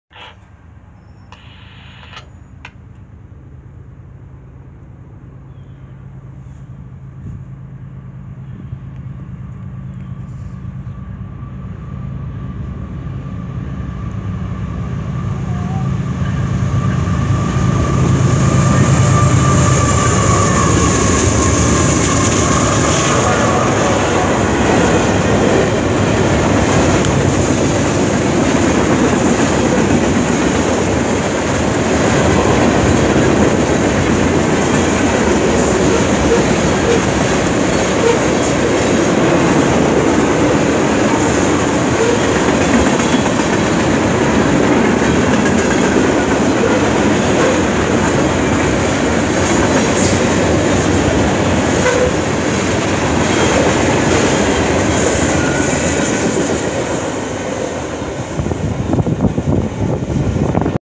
An eastbound manifest
In order, from first engine to last, NS ES44AC 8183, NS C40-9W 9962, UP C60ACs 6922 and 6923, both with doors open.